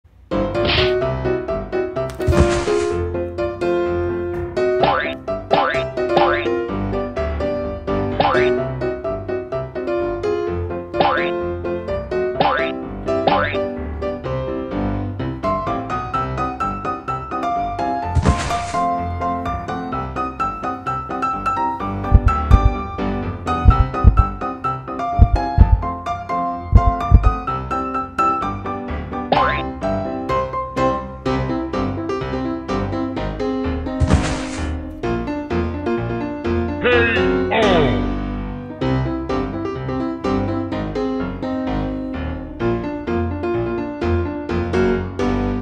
【腐宅喵日常】你看過海參翻身嗎？Yichi包惹怒女王Miku !
Yichi包6KG的海參身材能翻身能成功嗎？？

【Otaku Cat 腐宅喵】
佛系更新，一切隨緣